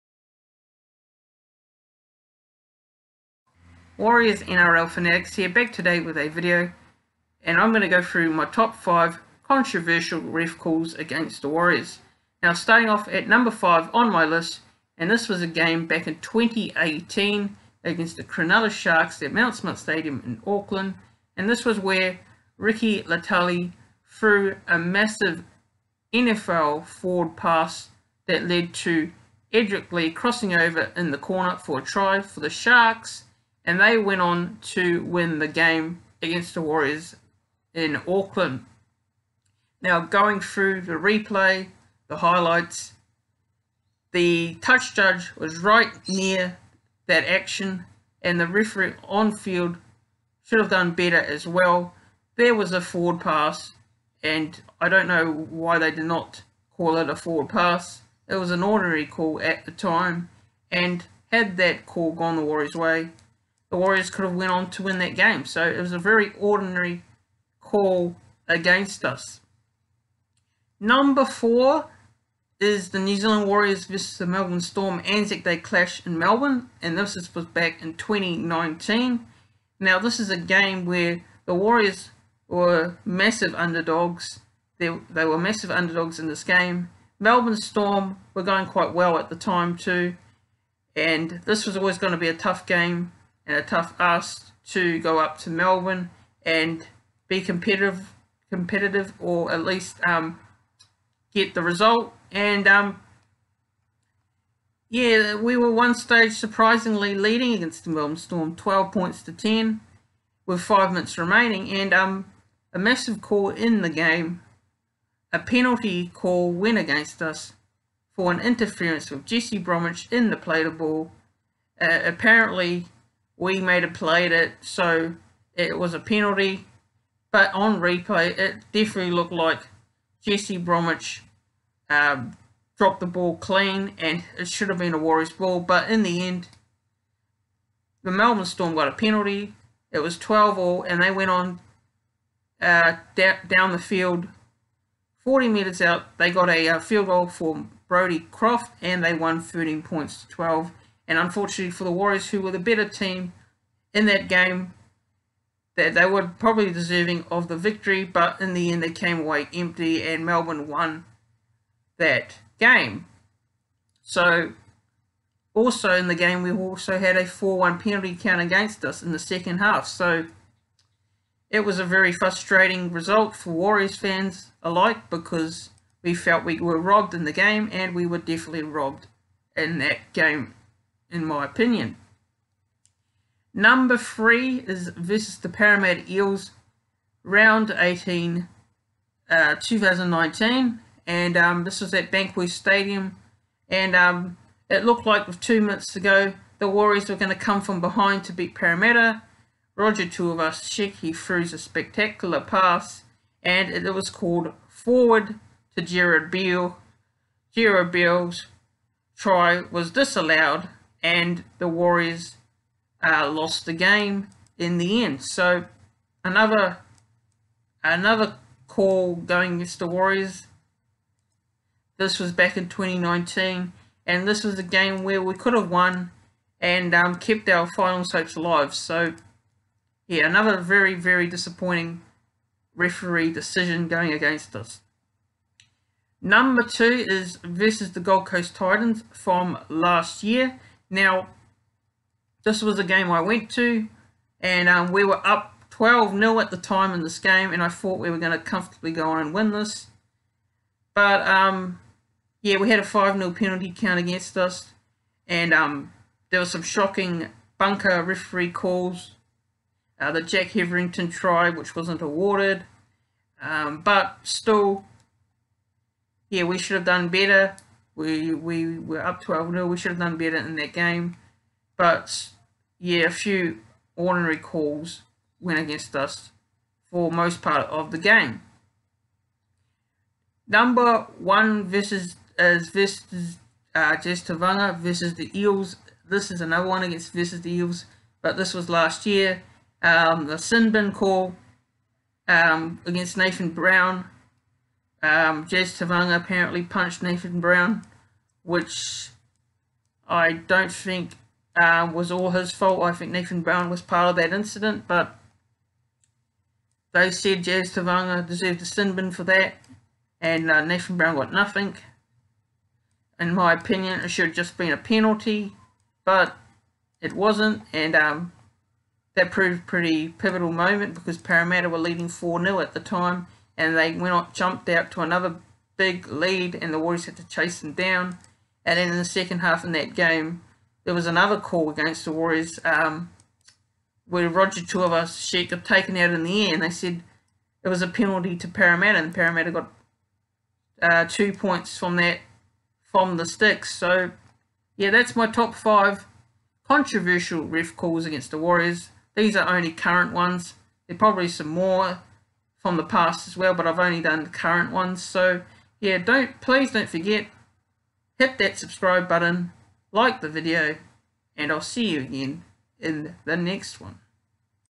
Top 5 Controversial Calls against Warriors
Today's video is Talking about Top 5 Controversial Calls against the Warriors